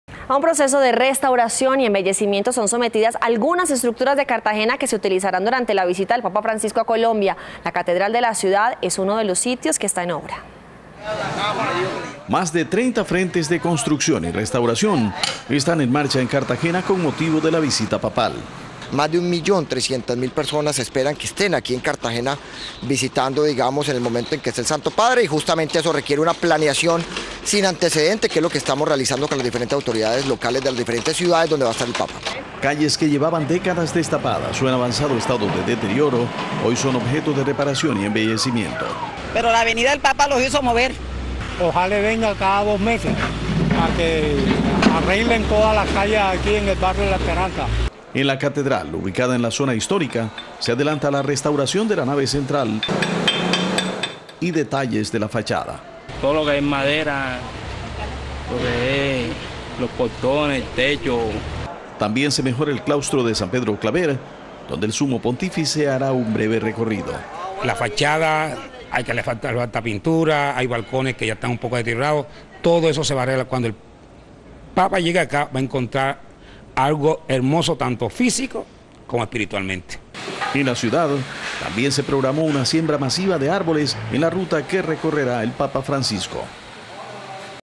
Ojalá el papa venga cada dos meses para que arreglen todas las calles: habitante de Cartagena
Emitido el 03 de Agosto de 2017. A un proceso de restauración y embellecimiento son sometidas algunas estructuras de la ciudad a propósito de la visita de Francisco.Más de 30 frentes de trabajo de construcción y restauración están en marcha en la heroica con motivo de la llegada del papa."Más de un millón 300 mil personas se espera que estén aquí en Cartagena en el momento que esté el santo padre", dijo Alejandro Maya, viceministro de Transporte.Calles que llevaban décadas destapadas o en avanzado estado de deterioro, hoy son objeto de reparación y embellecimiento.En la catedral, ubicada en la zona histórica, se adelanta la restauración de la nave central y detalles de la fachada.También se mejora el Claustro de San Pedro Claver, donde el sumo pontífice hará un breve recorrido.En la ciudad se programa, además, una siembra masiva de árboles en la ruta que transitará Francisco.